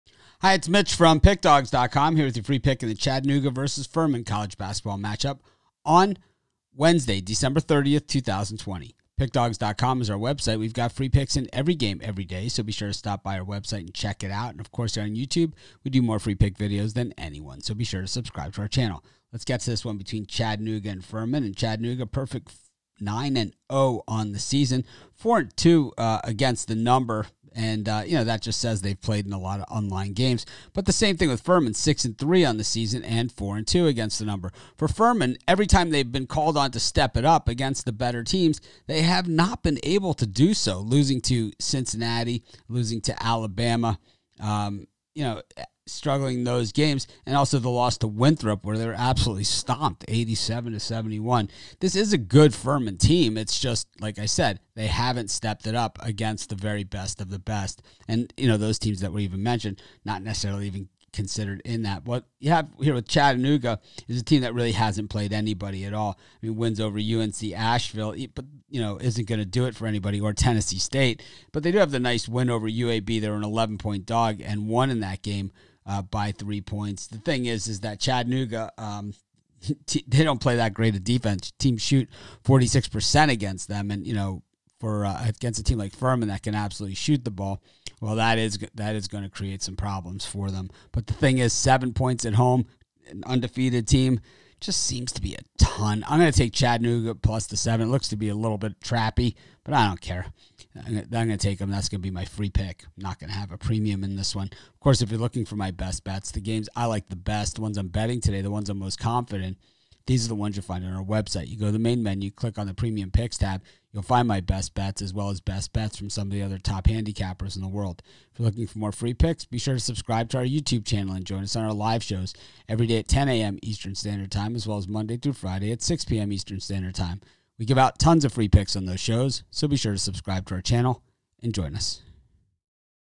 Chattanooga vs Furman 12/30/20 Free College Basketball Pick and Prediction CBB Betting Tips
The Furman Paladins take on the Chattanooga Mocs in Wednesday college basketball action. Get Mitch's College Basketball Pick and Prediction for this game.